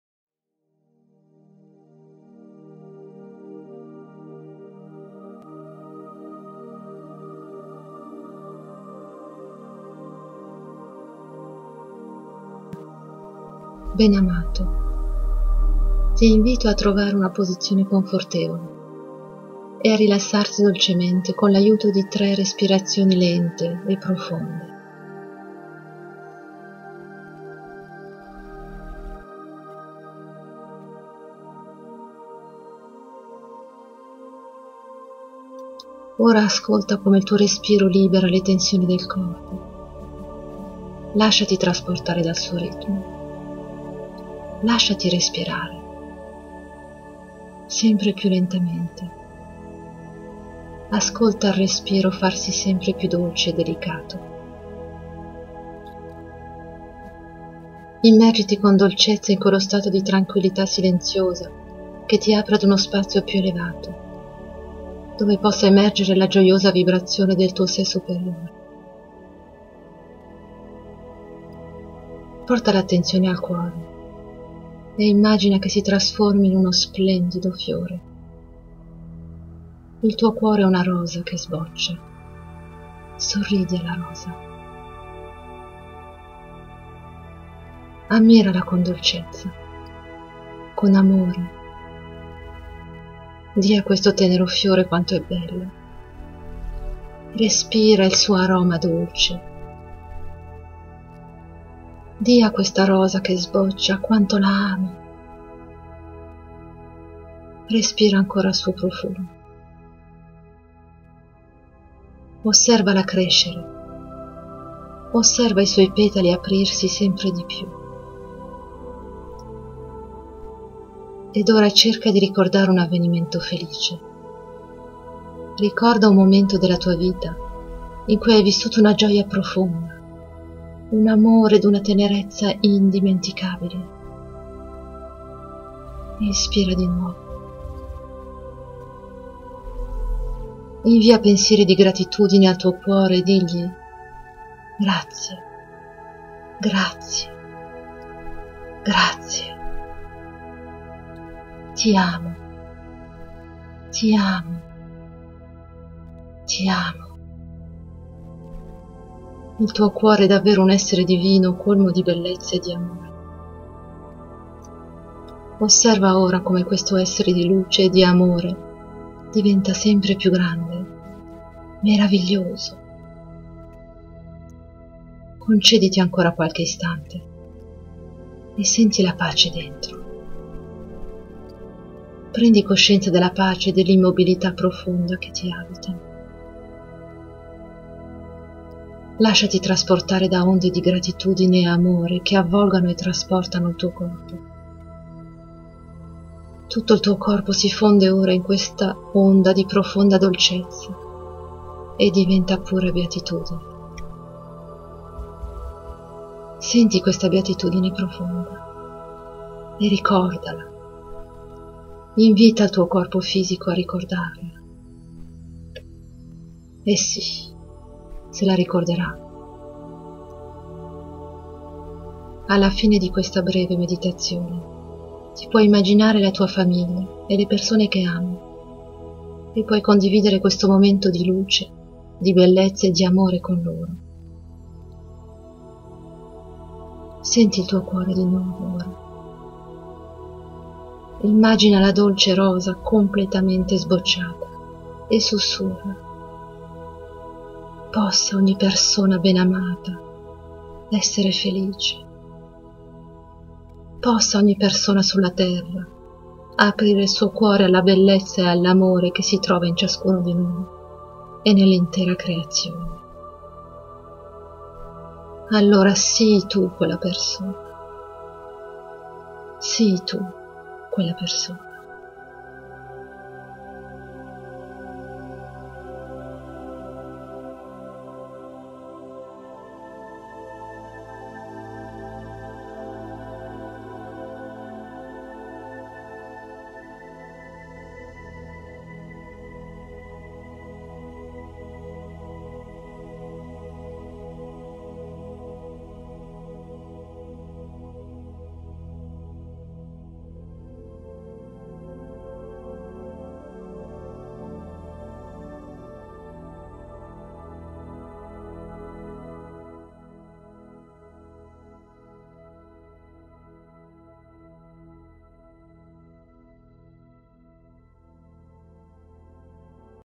Meditazione Della Rosa - con Chiara Vitali
Meditation of the Rose in Italian with the beautiful voice of Chiara Vitali.
A wonderful gift from the Great Ones.
Healing and soothing. Great for therapy work and relaxation.

Production: Yasen Nikolov